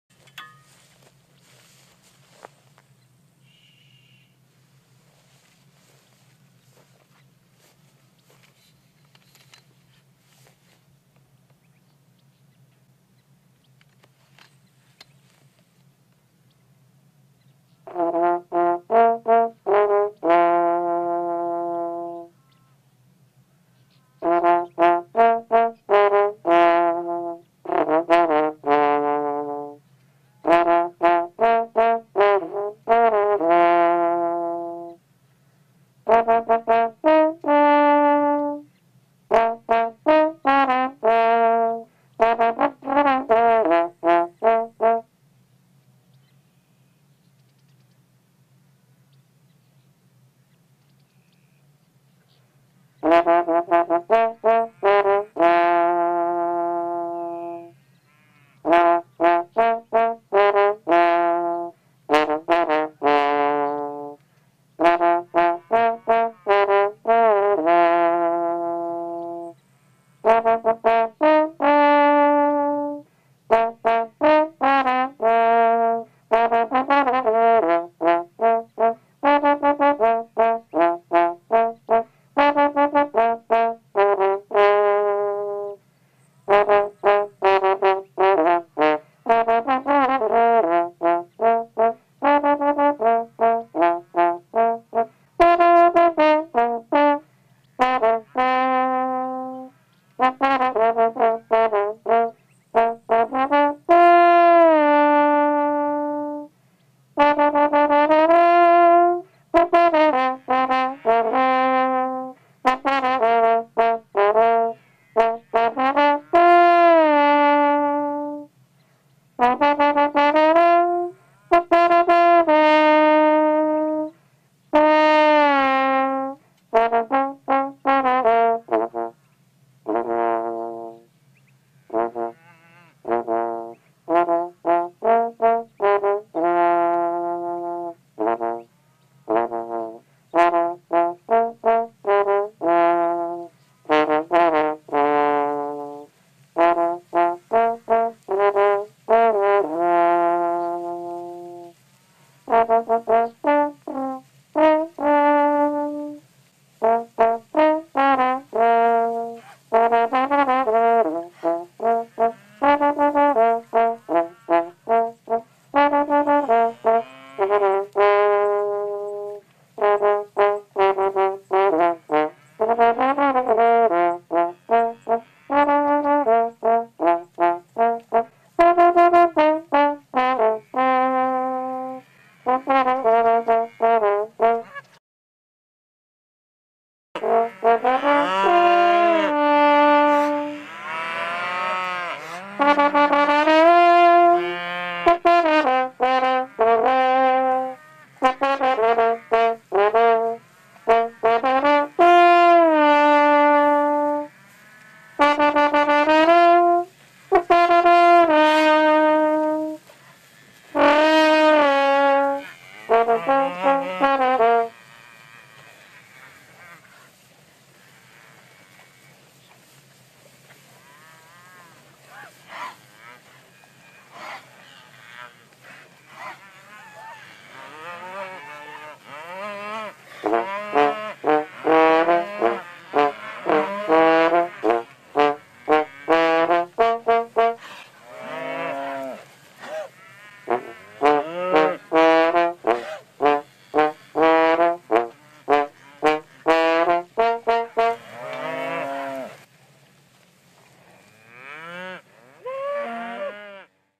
Unbelieveable ! Different ways to communicate
Look how he calls all the cows through music instrument